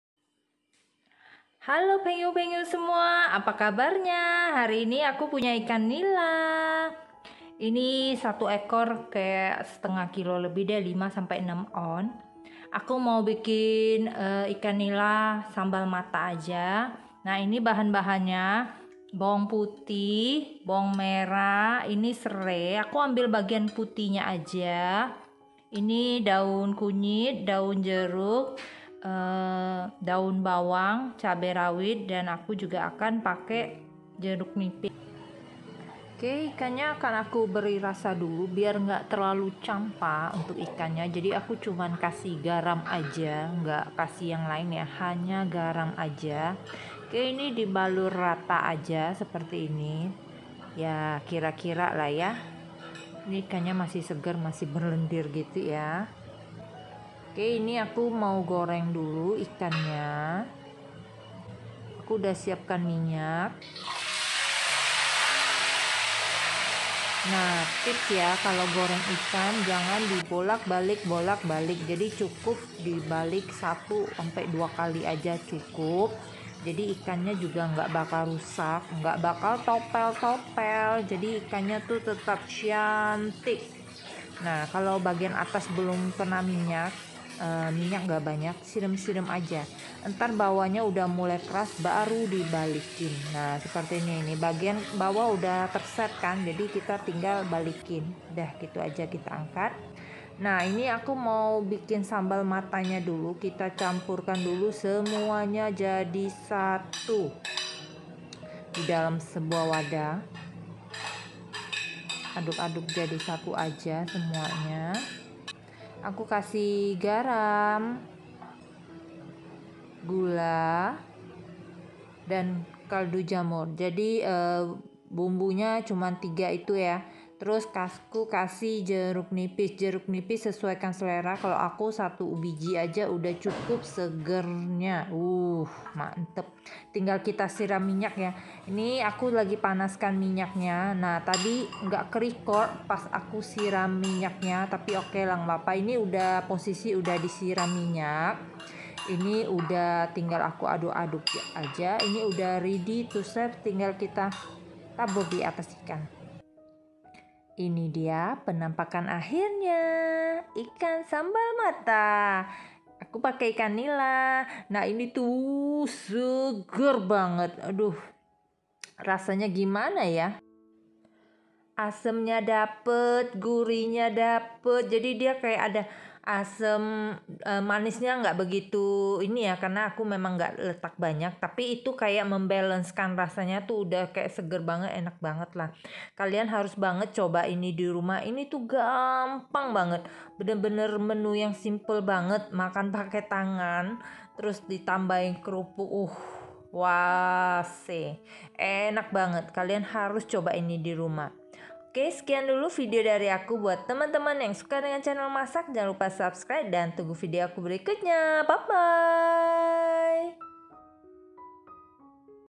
Ikan Nila Sambal Matah
Bahan :
1ekor ikan nila
3btg serai ambil bagian putih saja
6siung bawang merah
2siung bawang putih
1btg daun bawang
1lbr daun kunyit
8lbr daun jeruk
30gr cabe rawit setan
3-4sdm minyak goreng
½sdt garam
½sdt kaldu jamur
½sdt gula pasir